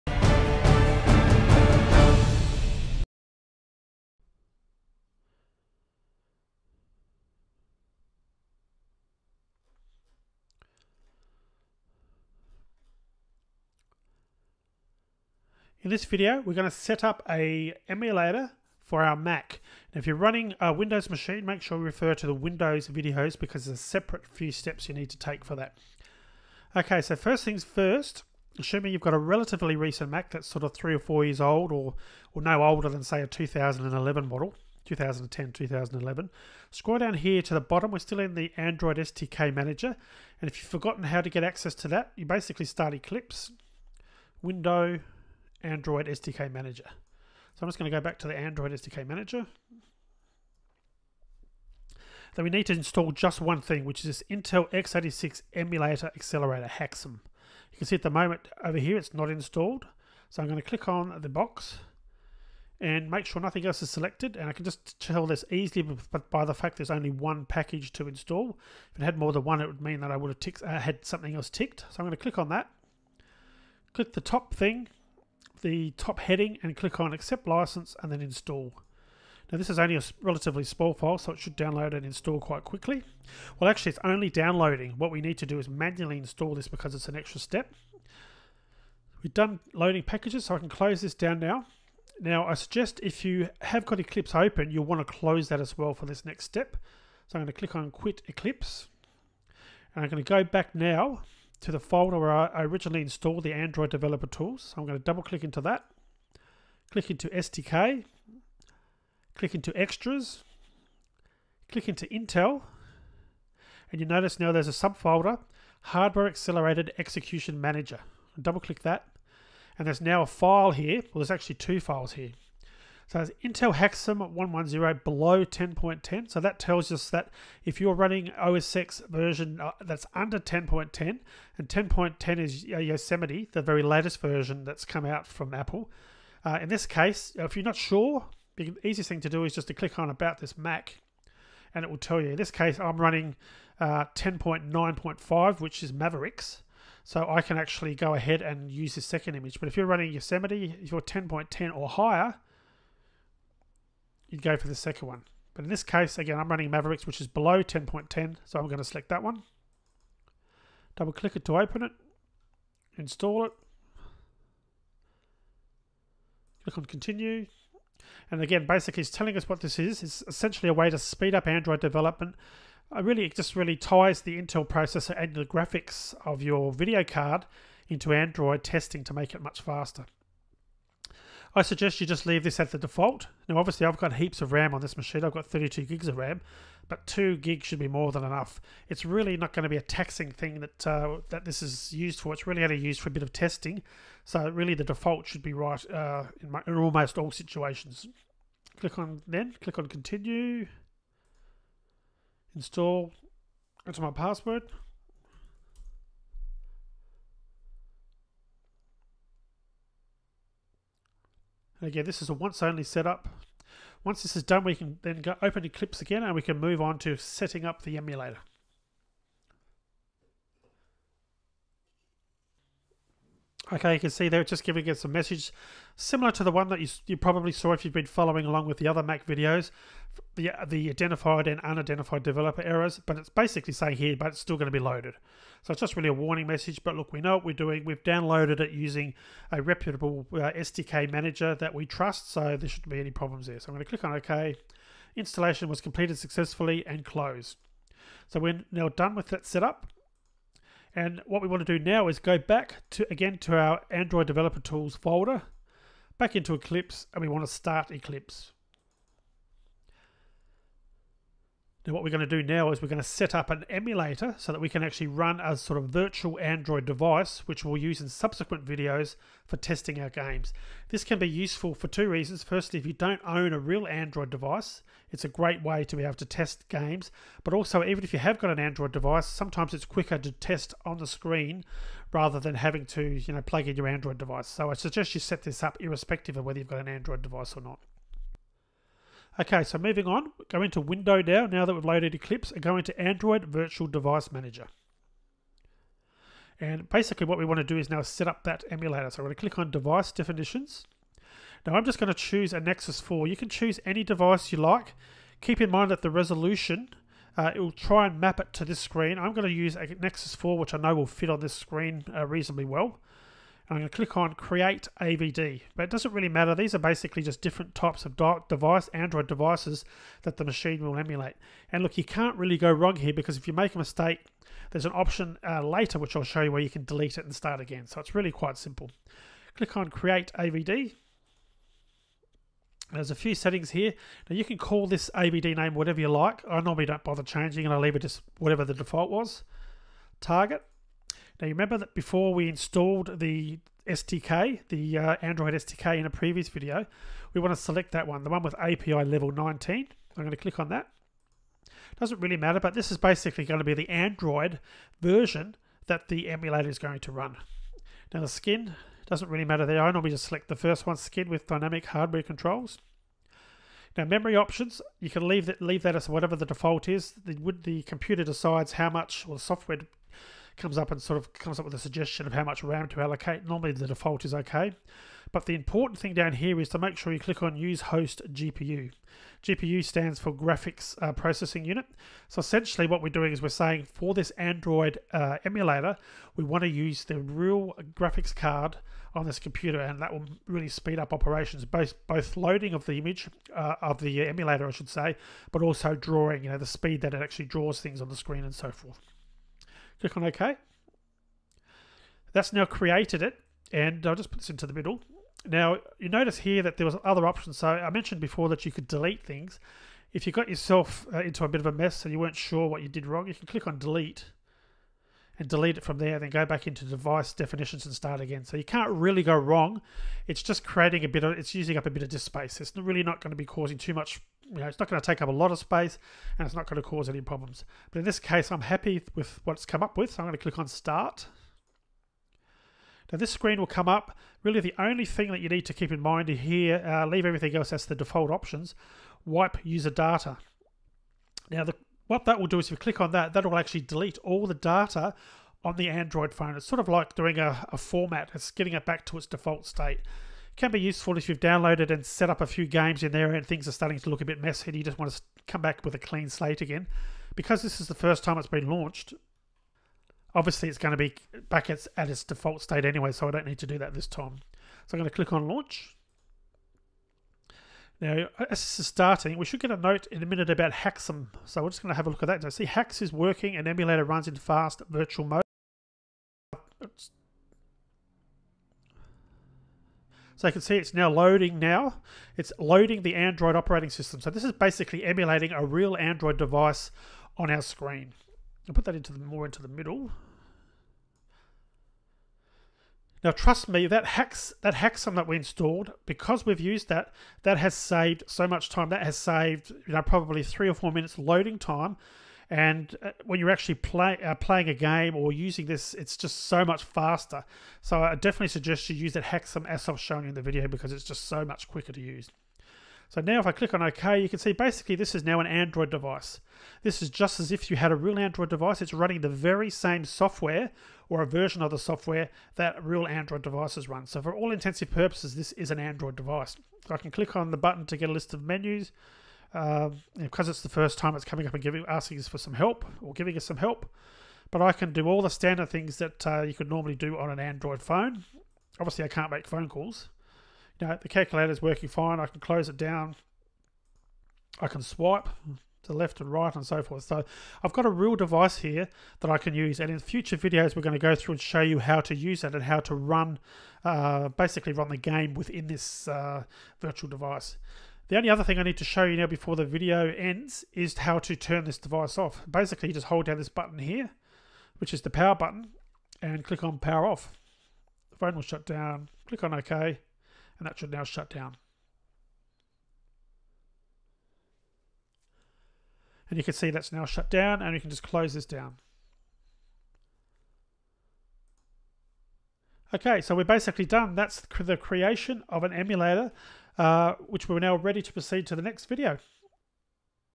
Android Virtual Device: Creating A HAXM Enabled Emulator With Eclipse
Test video showing how to setup an Android Virtual Device using the Intel Hardware Accelerated Execution Manager for faster performance, ready for use with Eclipse/Java.

My personal blog

avd, emulator, google, developer, tools, sdk, manager, mac, haxm, api, 19, android virtual device, android, virtual, device, eclipse